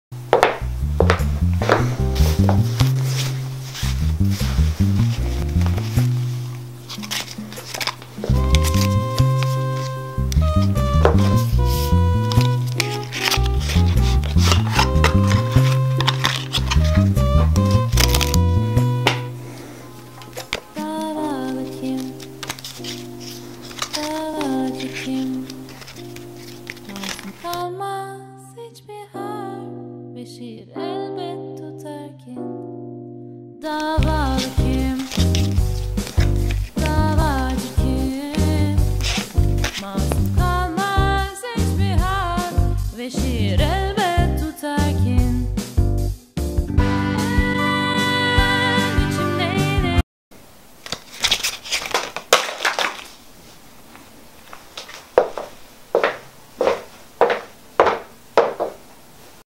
HalfS TEASER
Merhaba, ben Yiğit Yaşar.
Bugünkü videoda yakınlarda öğreteceğim bir hareket olan ve daha ismini koyamadığım hareketin teaserini çektim umarım beğenmişsinizdir : D